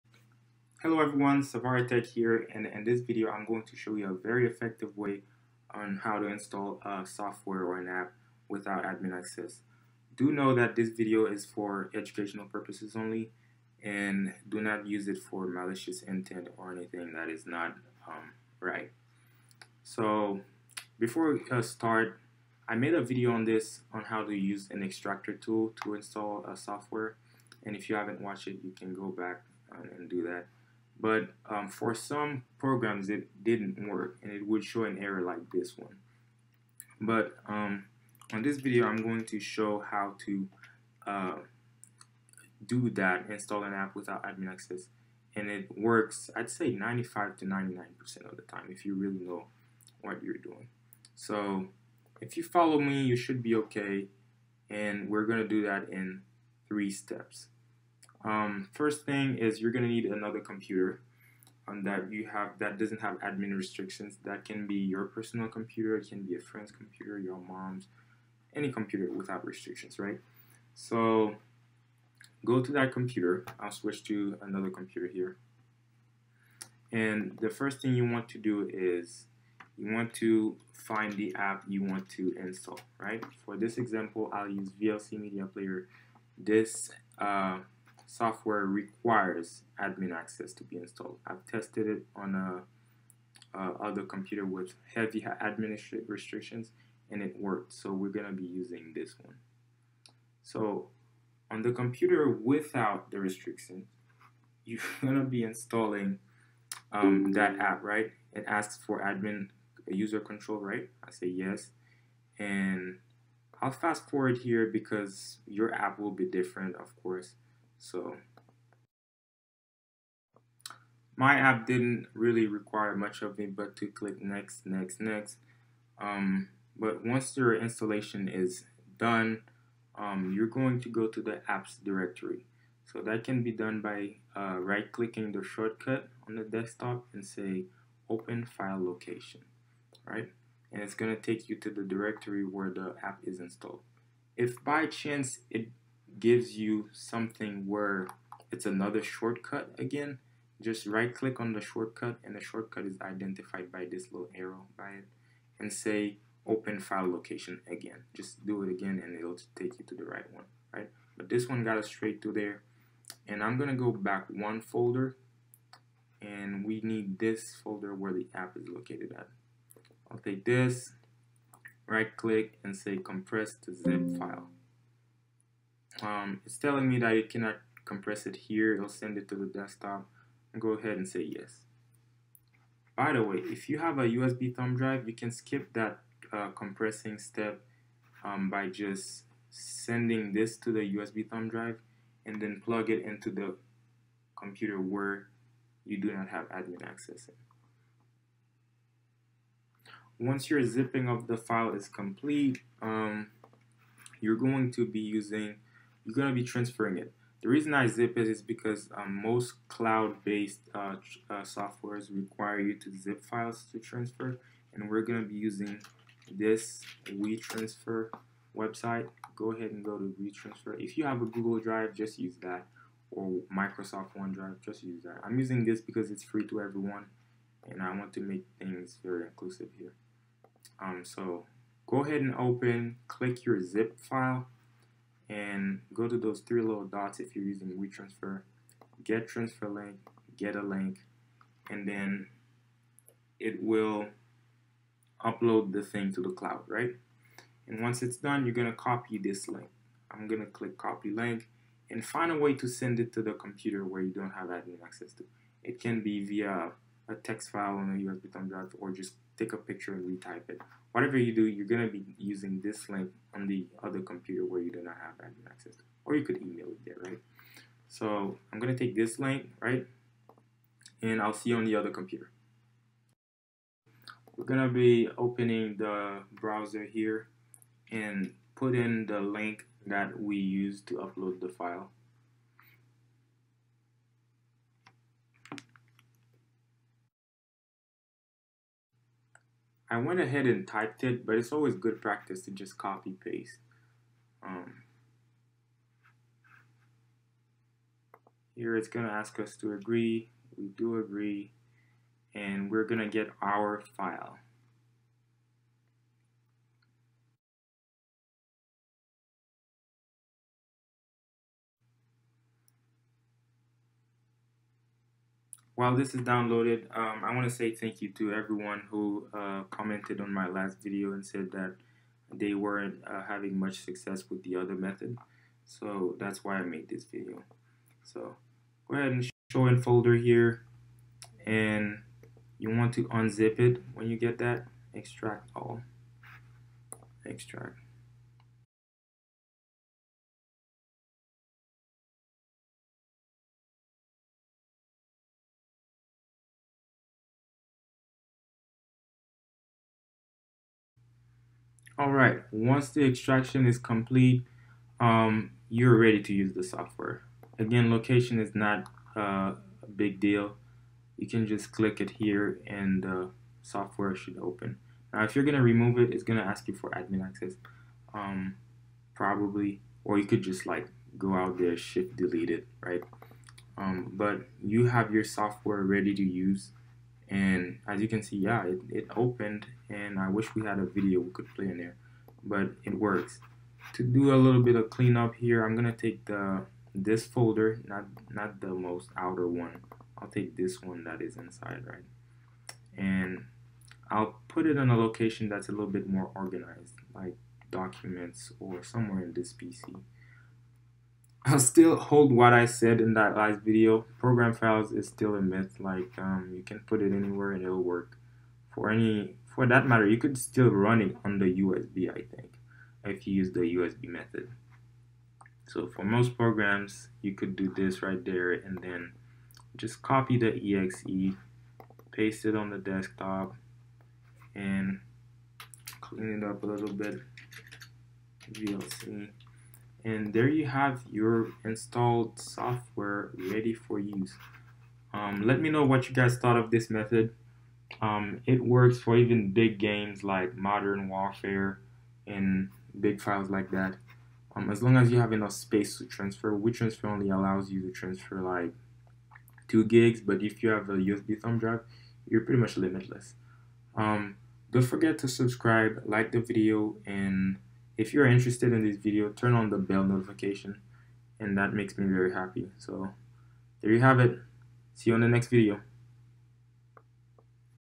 How to Install Apps Without Admin Access - A Better Way
This is one way people can install apps/programs without Admin permissions

IT People📣, Be mindful that you might want to do more than stop users from running apps as an Admin

Regular Users📣, Use this if you lock yourself out, if it is unnecessary to contact IT, or if it is allowed by your organization/school's policy

0:00 - Intro & Disclaimer
1:00 - Getting & Preparing App for Install
3:35 - Transferring App
5:25 - Extracting & Launching App
7:00 - Cleaning Up